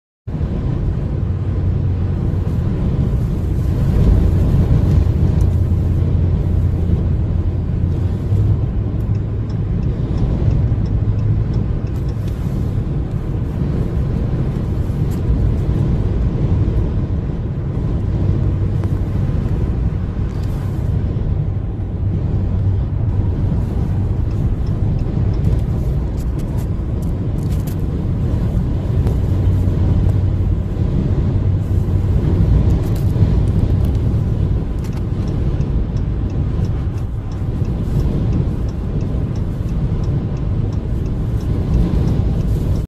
Dashcam Footage To
A Wind Strucks a Semi Truck At Detroit, Michigan

Note: Don't Copyright This Video, I Swear